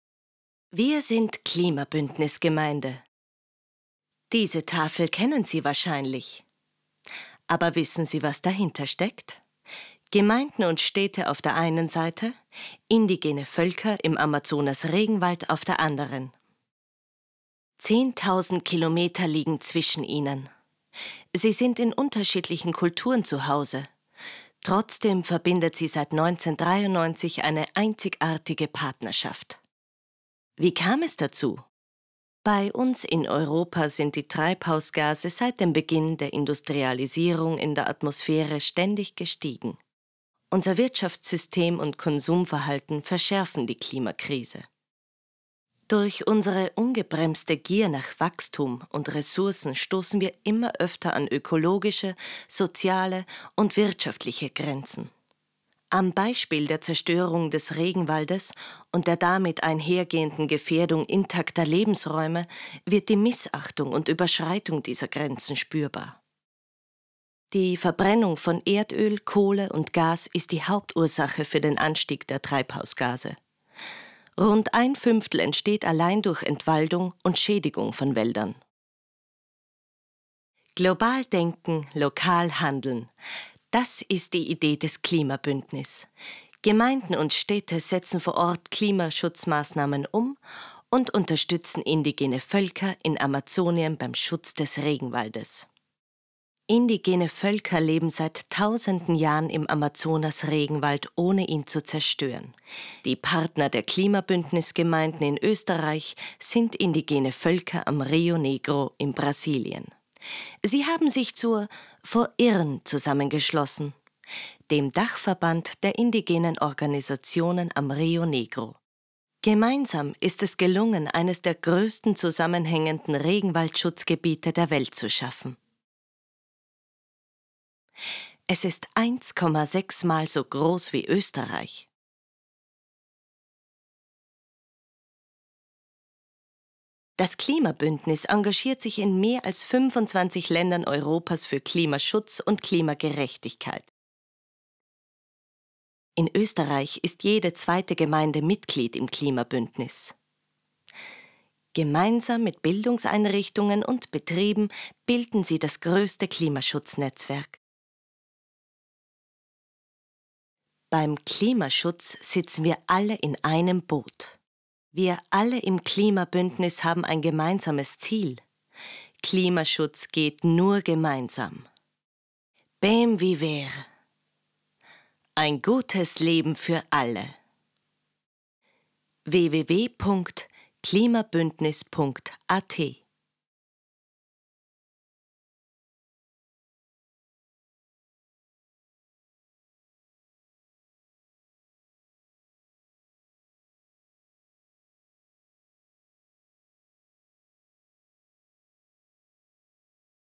Wir sind Klimabündnis-Gemeinde
Was steckt hinter der Tafel Klimabündnis-Gemeinde?
Was verbindet Klimabündnis-Gemeinden mit indigenen Völkern?
Und was ist die Idee das Klimabündnis?
Ein Kurzfilm über das größte Klimaschutz-Netzwerk Österreichs.